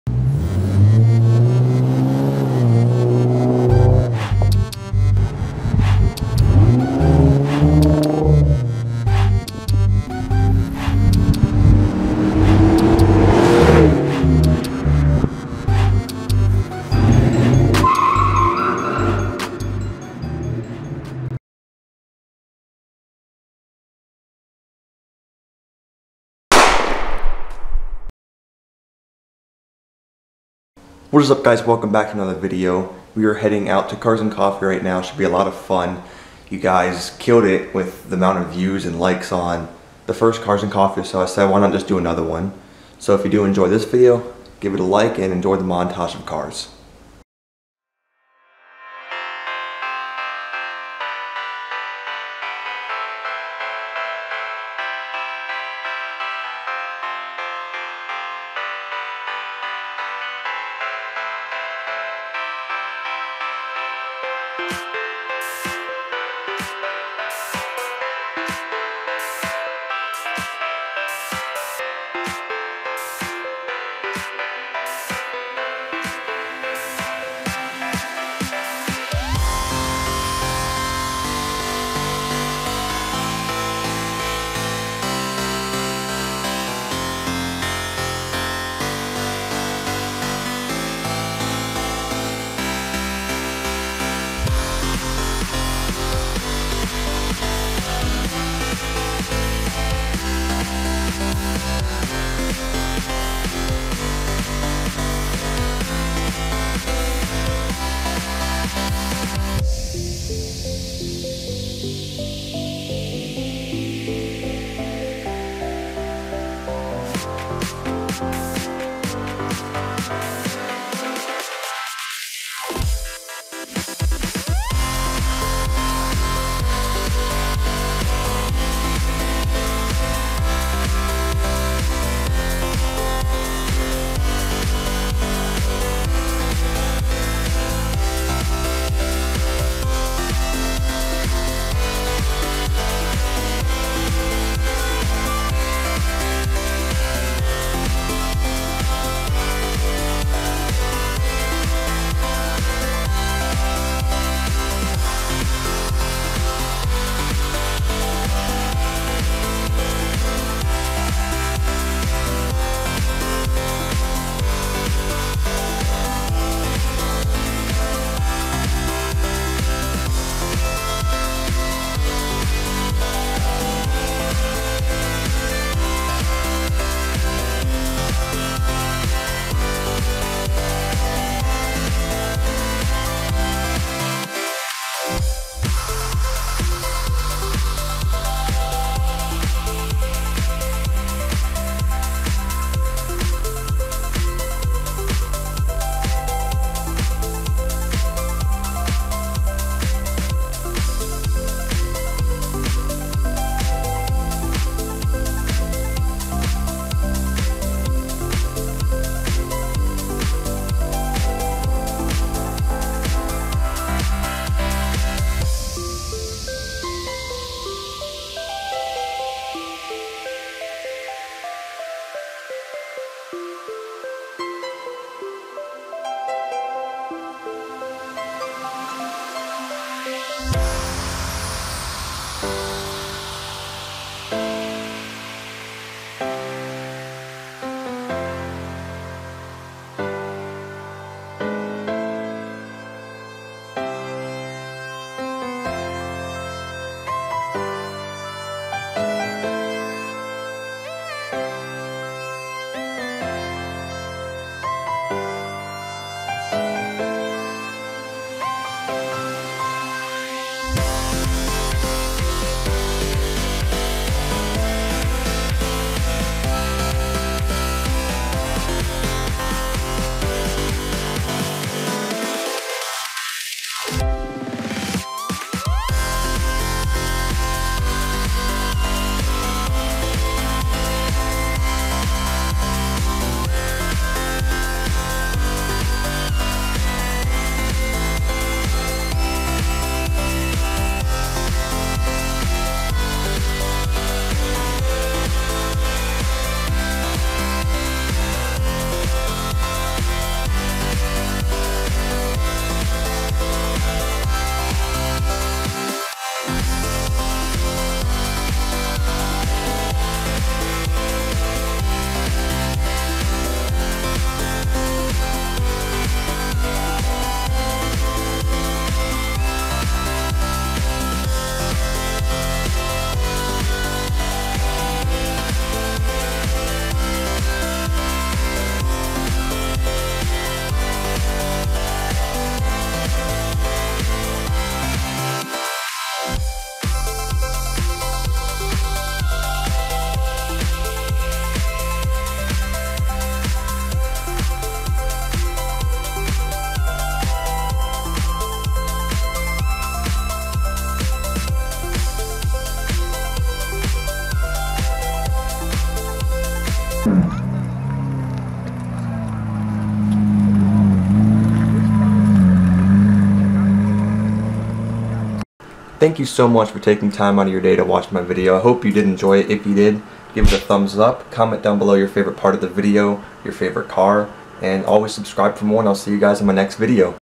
CARS AND COFFEE #2!! | Mustang Edition!
You guys seemed to love the last cars and coffee and I loved making the video so here's another one! Can we hit 3 likes!??! Thanks!
GT Mods: Flowmaster Original 40 Mufflers, Wheels and Tires, Chin Spoiler, Headlights, and Decklid!
V6 Mods: Pypes Mid-Muffler True Dual Exhaust, Wheels and Tires, Slotted and Drilled Rotors, Headlight/Tail Light Covers, GT Style Grille, and Decklid!